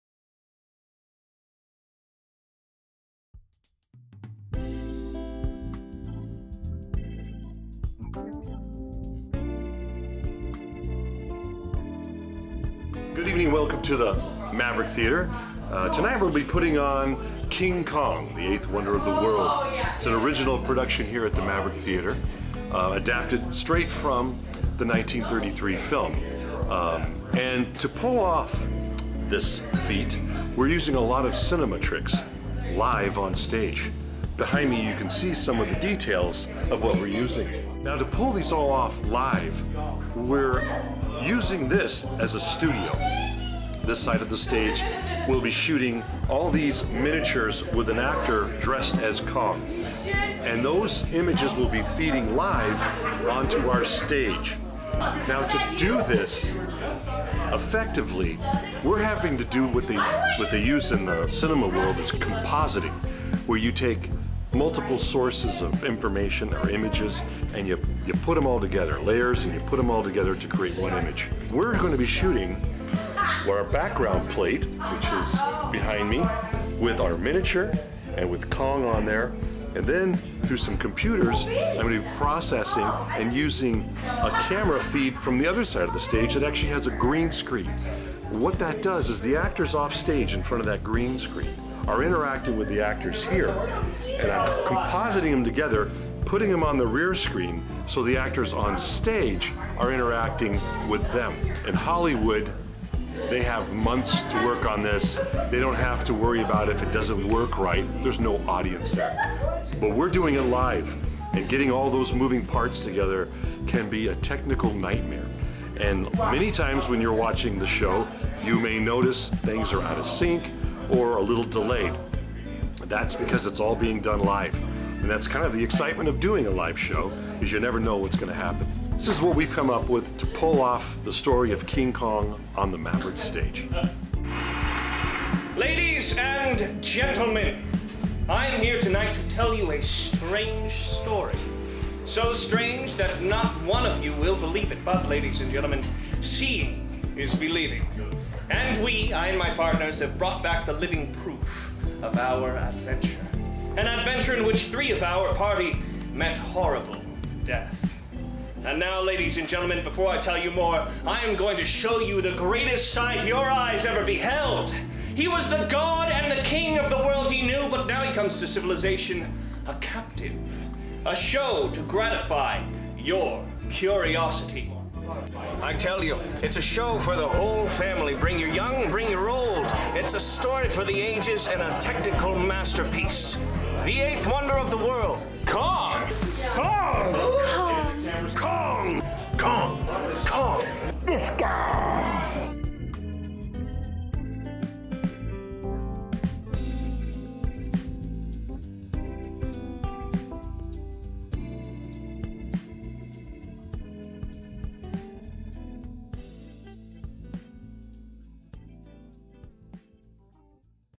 "King Kong" at Maverick Theater
2021 features the return of the eighth wonder of the world to Fullerton's Maverick Theater. "King Kong" runs weekends through Sept. 11, 2021.